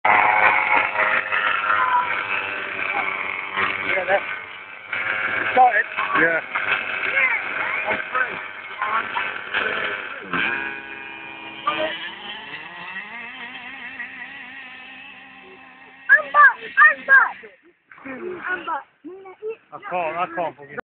FG BAJA O.B.R.27cc LR,FM. vs HPI BAJA STD 27cc CY 4 BOLT. DRAG RACE ON GRASS
JUST A LITTLE RACE TO SEE HOW QUICK THE BAJA'S ARE.